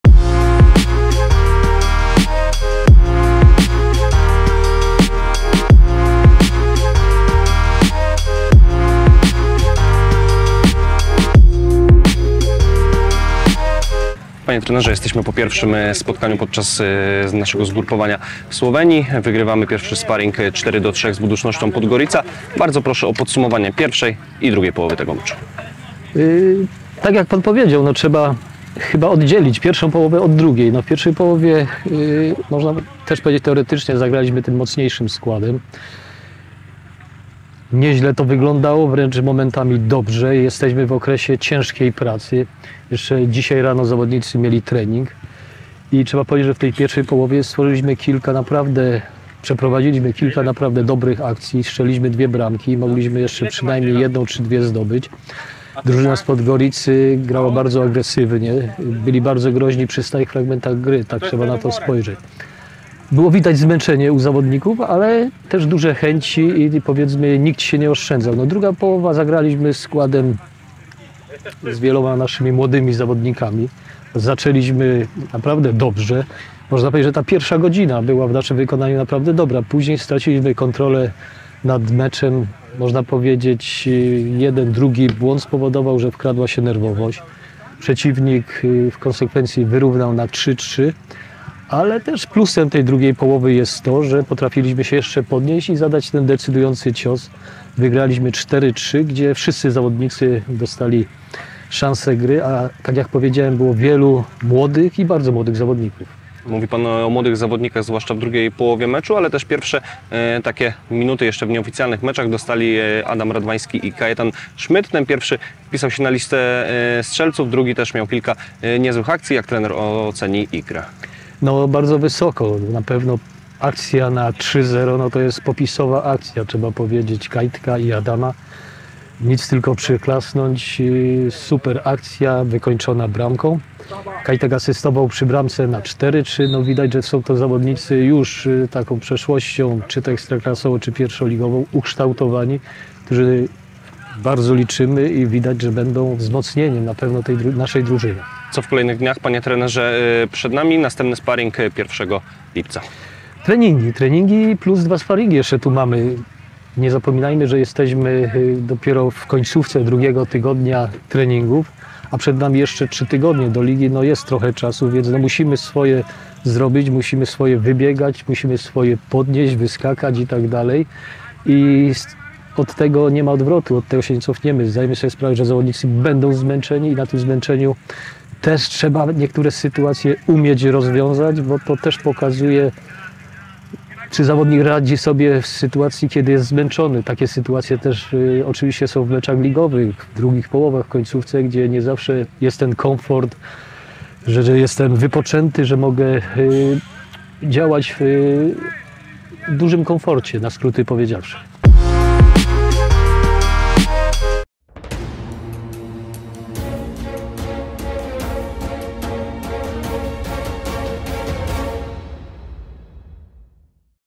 KGHM Zagłębie Lubin 4:3 Buducnost Podgorica | Skrót meczu
Zapraszamy do objerzenia najciekawszych akcji pierwszego sparingu KGHM Zagłębia Lubin w słoweńskich Moravskich Toplicach. Gole w dzisiejszym meczu strzelili Tomasz Pieńko, Dawid Kurminowski, Adam Radwański i Arkadiusz Woźniak.